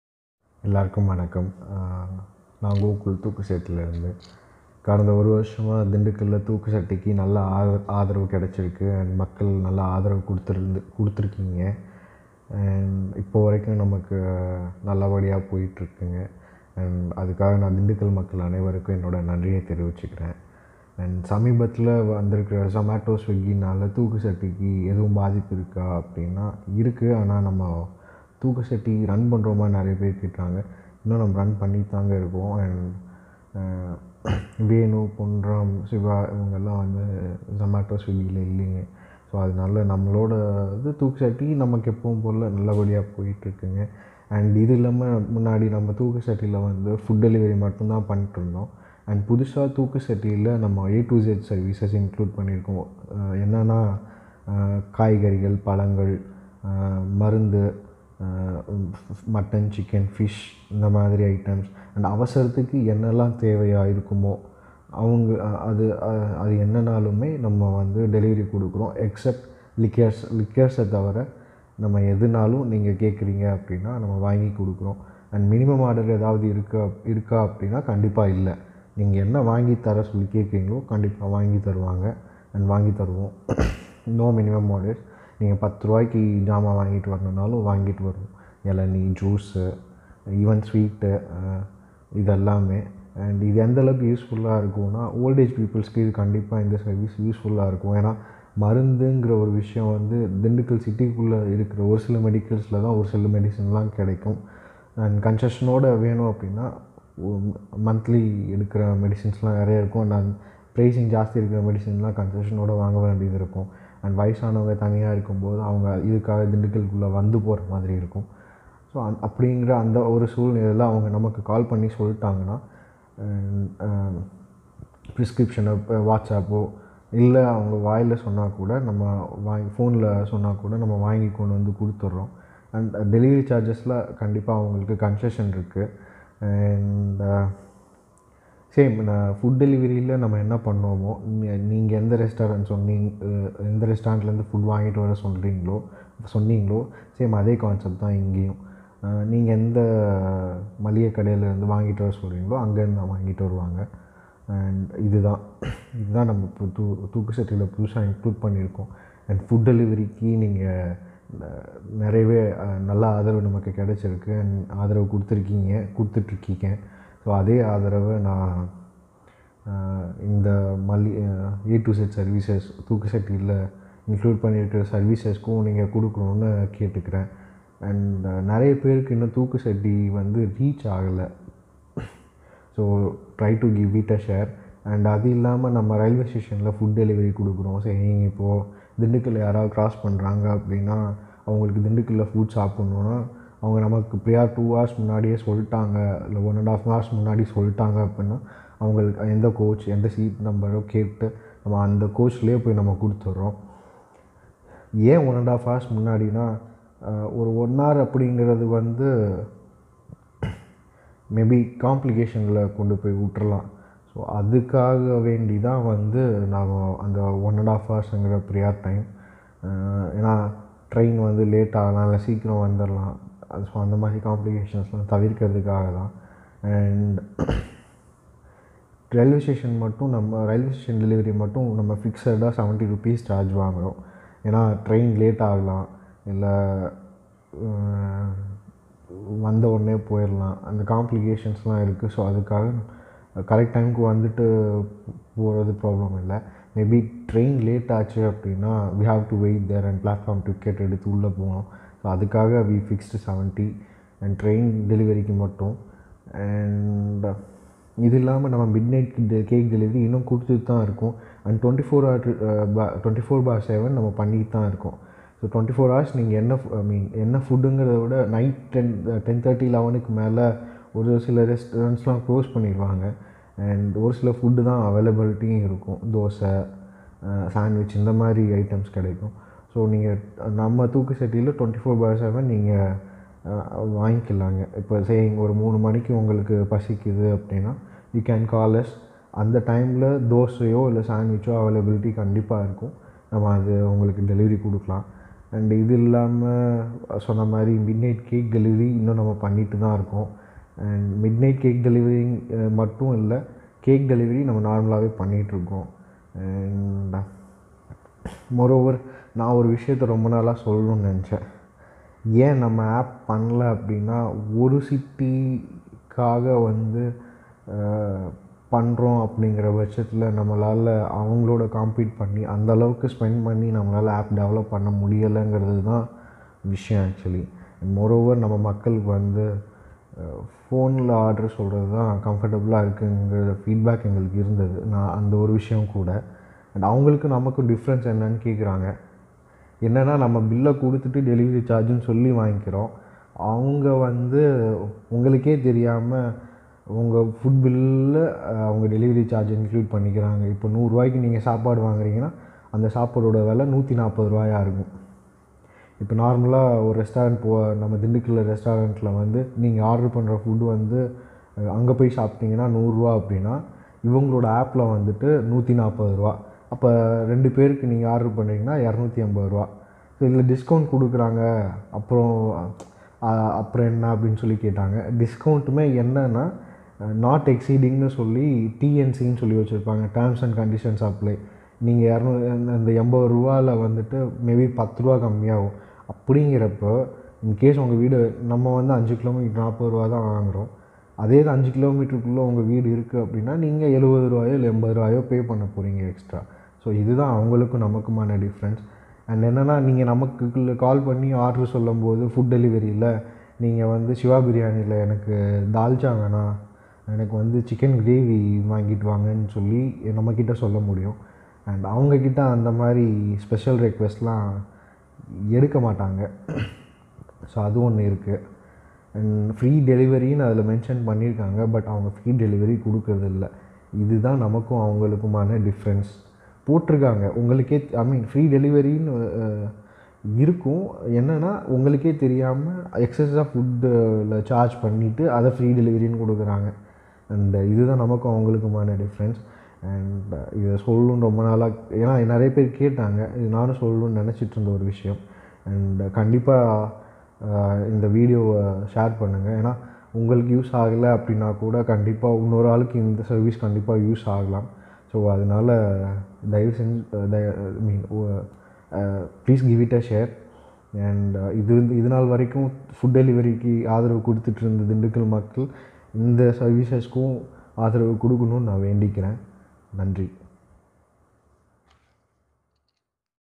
Thooku Satti-Food|Grocery|Vegetables&Fruits|Medicine Delivery in Dindigul
Now Thookusatti Has included Food,Cake,Grocery,Vegetables,Fruits & Medicine delivery in Dindigul,Now U call us & Order Anything you Want & Receive with in Mins...

In this video,spoke about
*Atrocities of Zomato & Swiggy
*Support local business
*Benefits of Supporting Local Business.